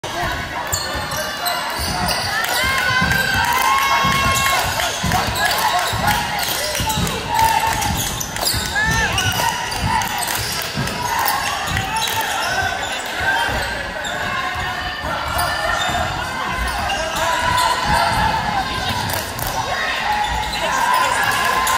Max Gonzalez c/o 2024 Highlight - District Game
Lake Worth Christian vs. Atlantic Christian December 13, 2022. Jersey #2, white team.